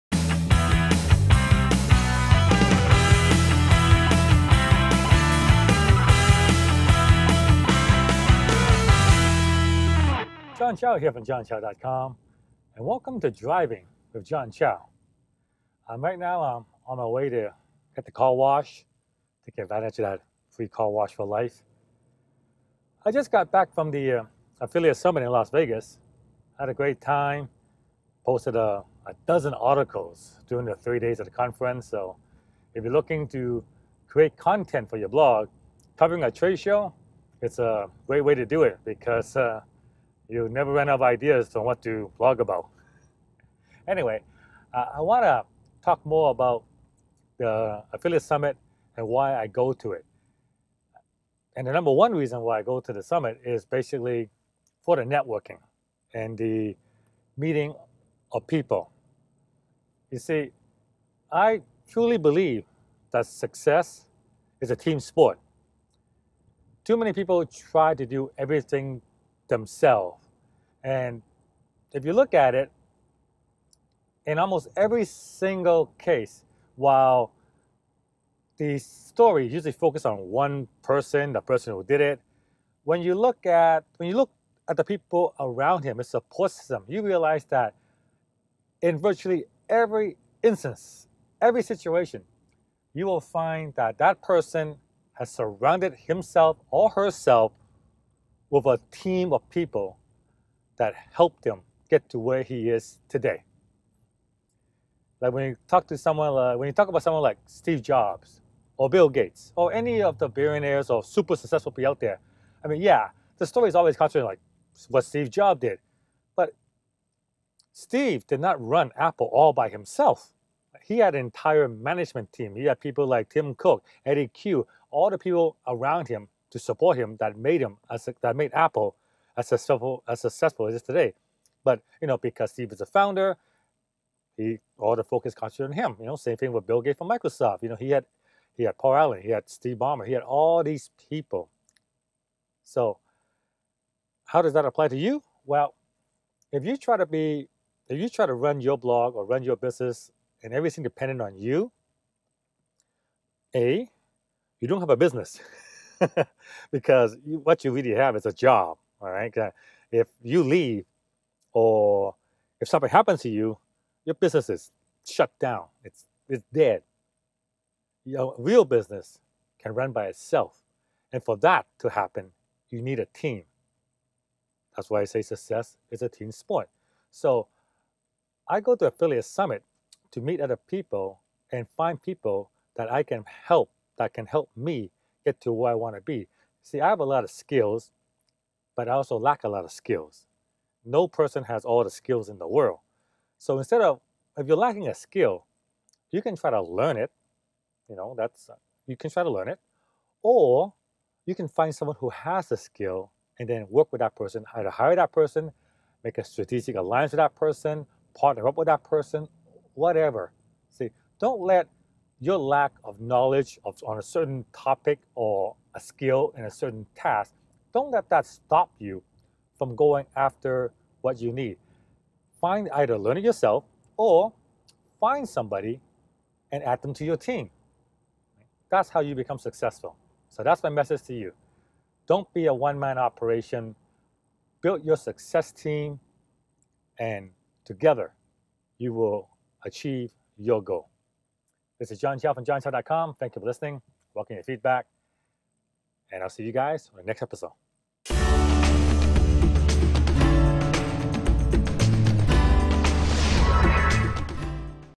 Drving with John Chow - Success Is A Team Sport
On this episode of Driving with John Chow, I explain why you can't be successful all by yourself. Success is a team sport.